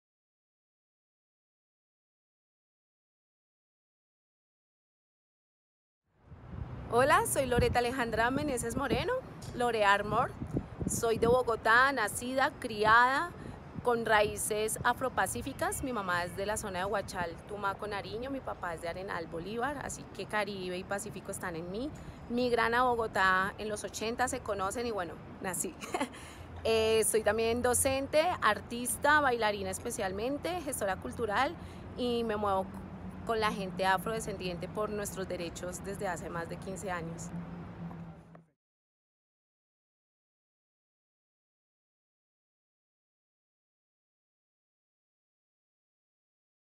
EPT: Presentación / Introducing Loretta Meneses
Loretta Meneses se presenta a la comunidad de EPT. Si quieres conocer la Anécdota de Pelos de Loretta sigue este enlace

Loretta Meneses introduces her self to the EPT community. If you want to watch her hair-anecdote follow this